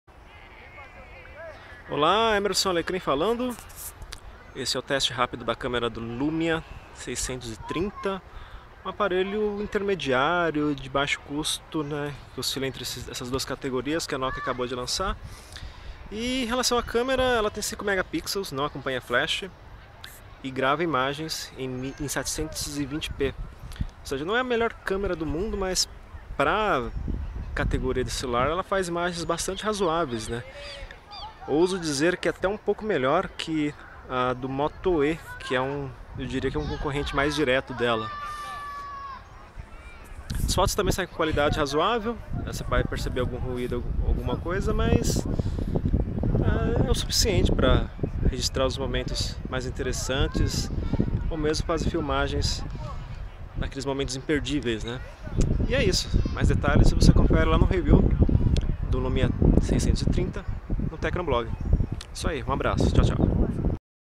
Teste de câmera - Nokia Lumia 630
Filmagem feita com o Nokia Lumia 630, com resolução 720p. O review completo está no Tecnoblog